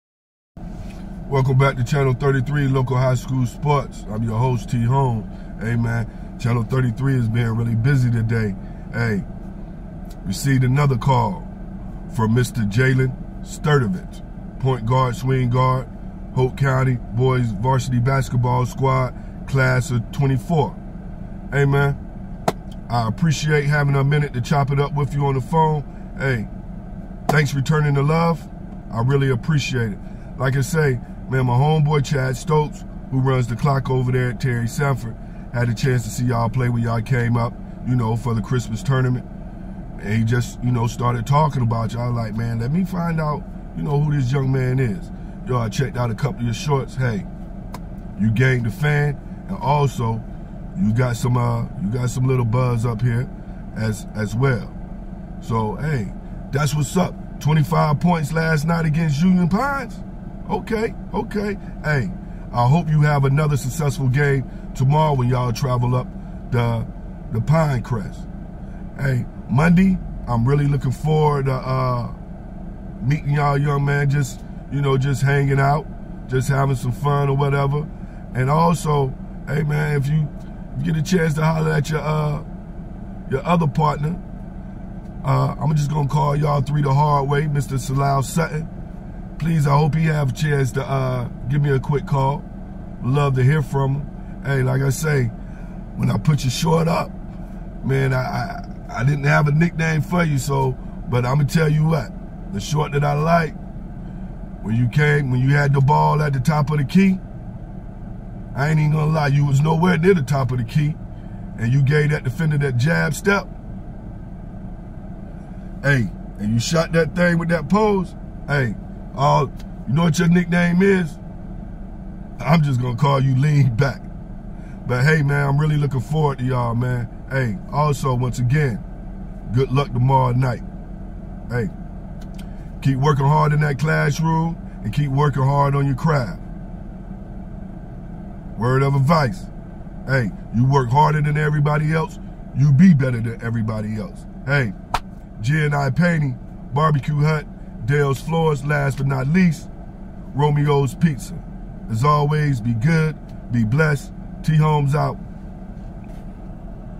C/O 24 Jaylen Sturdivent Hoke County HS, Raeford NC. Thanks for the call 25pts @ Union Pines 1/17/24
Phone call with Jayden Sturdivent Hoke County HS Basketball